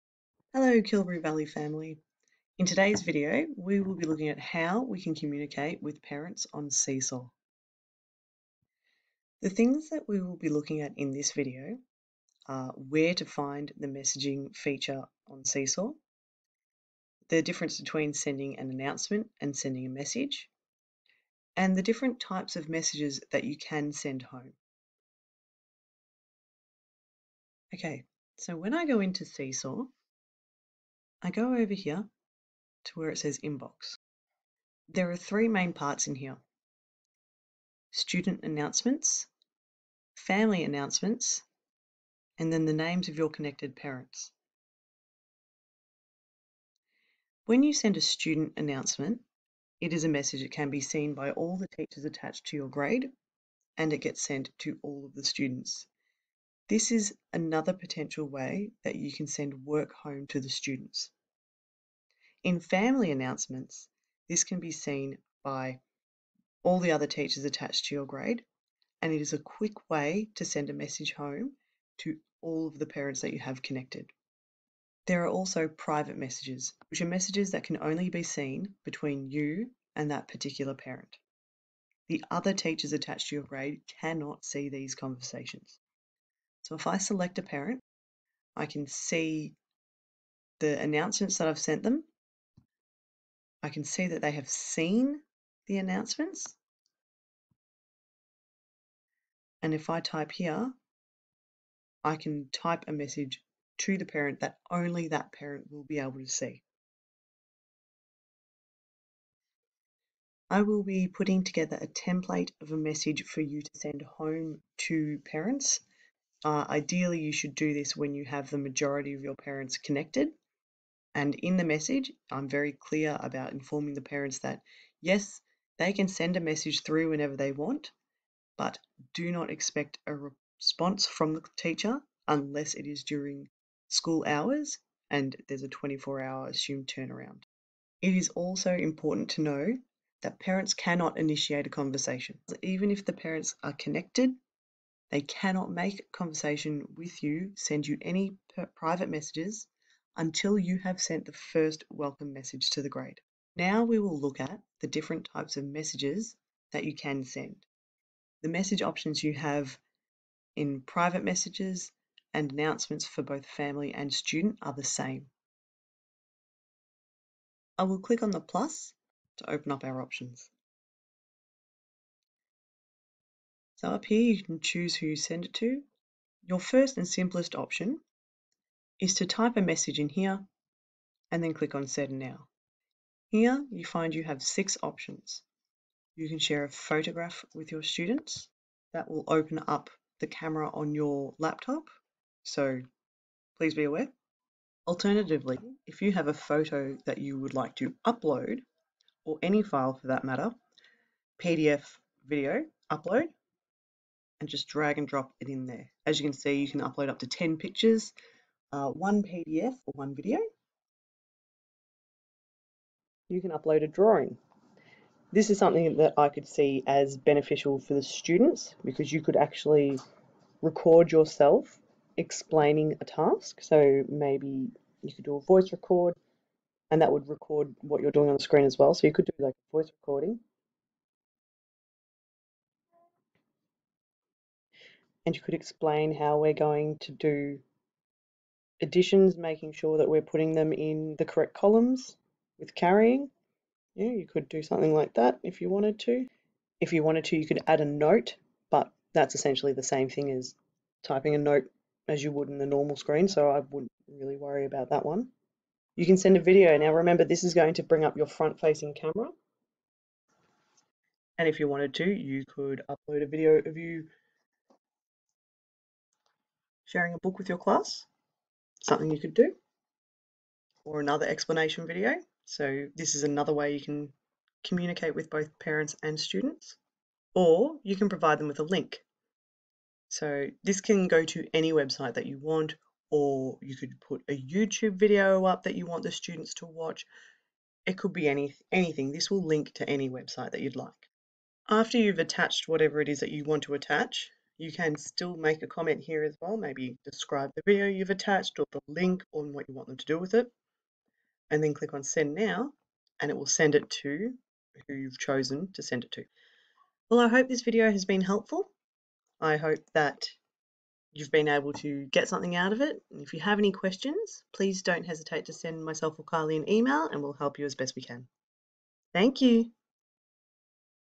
TEACHERS SEESAW Communicating with Parents
How to use Seesaw for communicating effectively with parents and students.